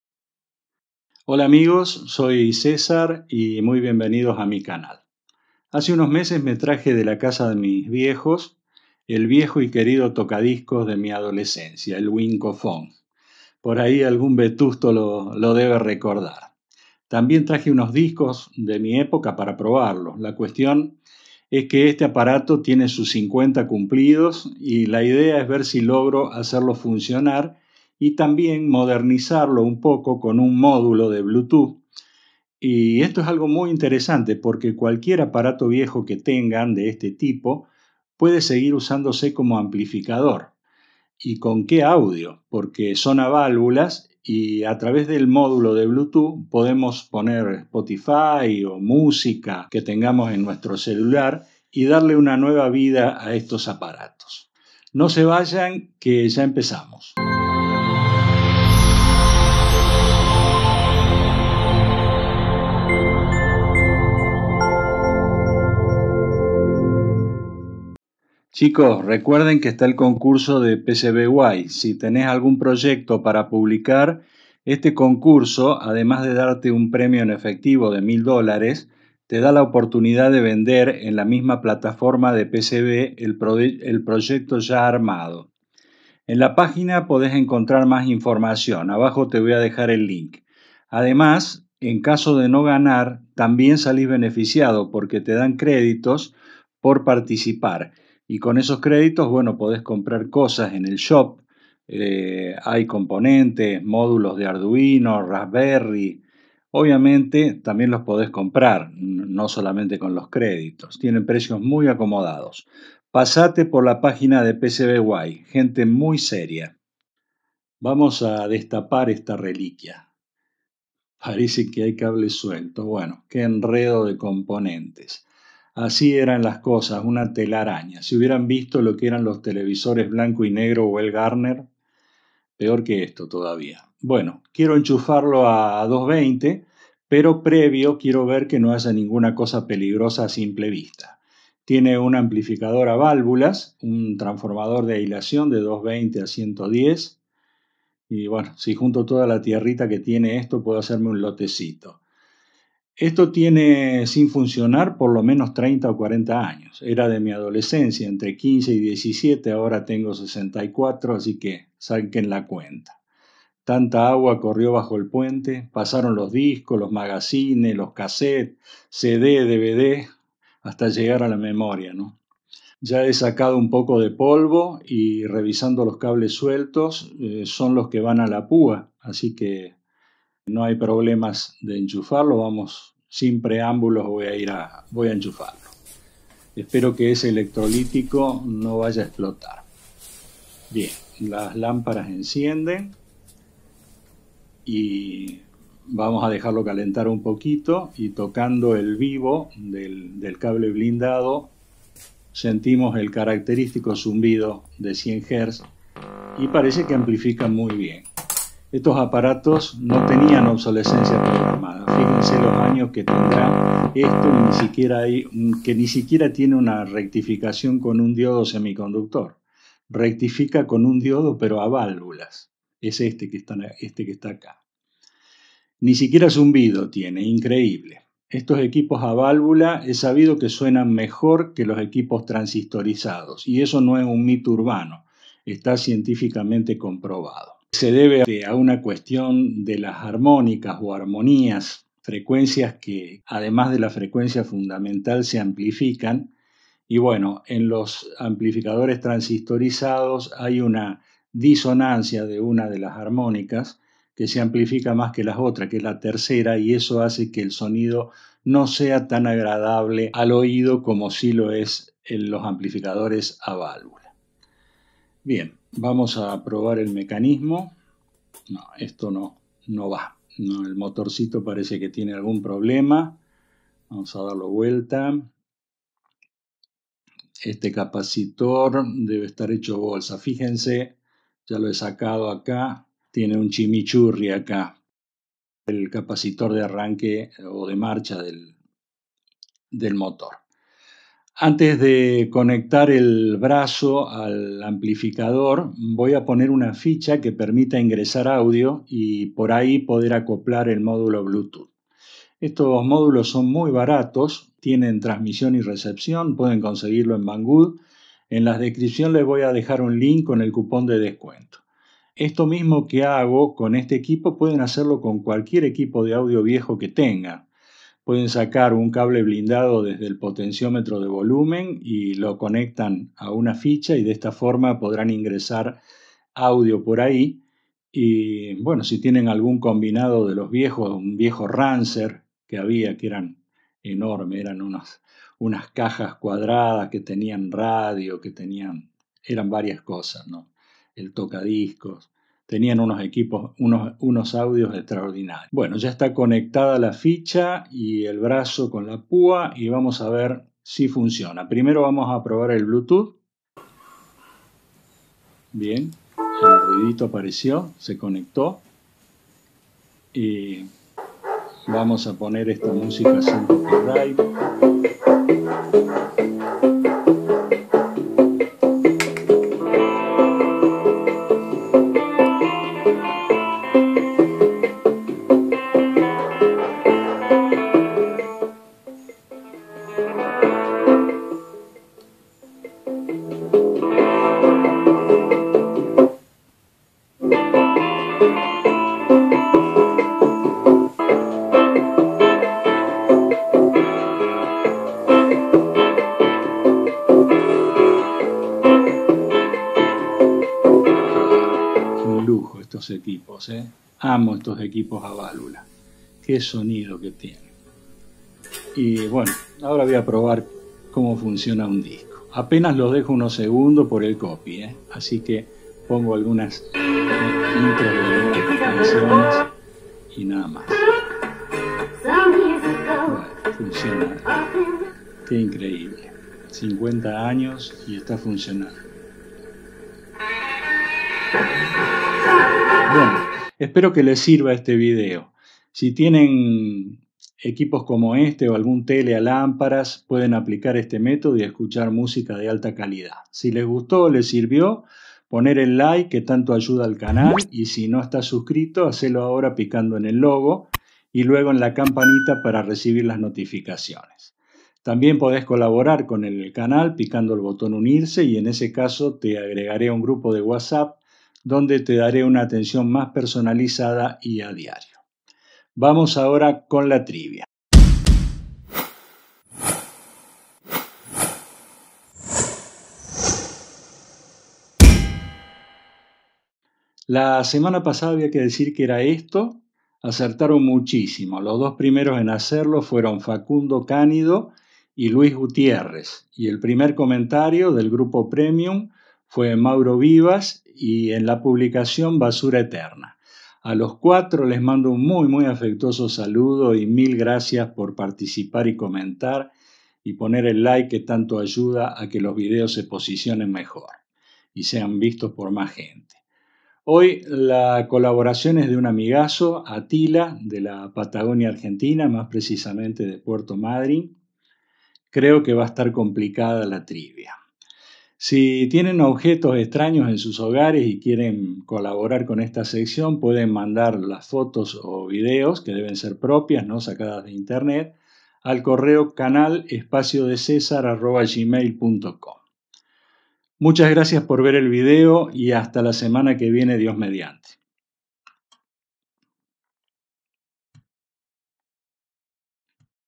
Como modernizar un tocadiscos wincofon de hace 50 años. Modulo Bluetooth
Comprar Modulos bluethooth en Banggood cupon es BGKN319 8.99$
( Cupon codigo es BG3f4da7 15% oferta para electrónica ,Herramientas, industriales y científicas)
En este video veremos como se pone en marcha un viejo tocadiscos de los 70 y se lo moderniza colocandole un modulo bluethoot que permite el uso del amplificador valvular. Este mismo procedimiento puede usarse con cualquier aparato viejo, combinado, televisor etc. es muy simple. No te pierdas el video